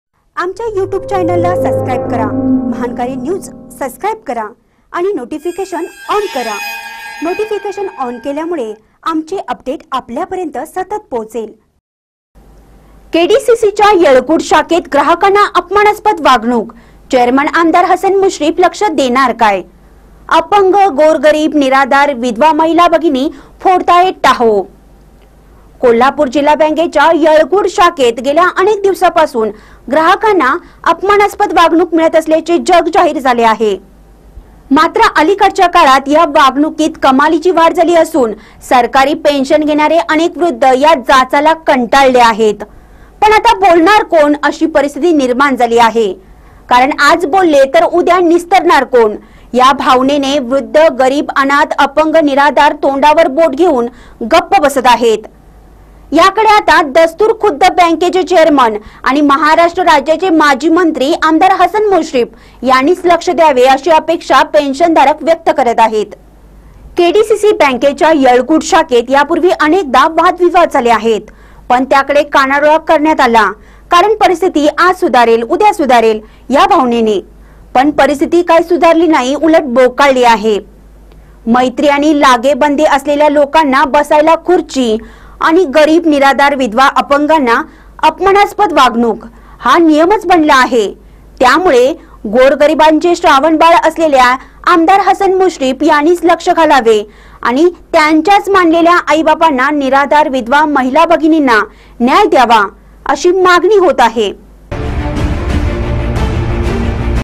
के डी सी सीच्या यळगूड शाखेत ग्राहकांना अपमानास्पद वागणूक
के डी सी सीच्या यळगूड शाखेत ग्राहकांना अपमानास्पद वागणूक ; चेअरमन  आ. हसन मुश्रीफ लक्ष देणार काय ?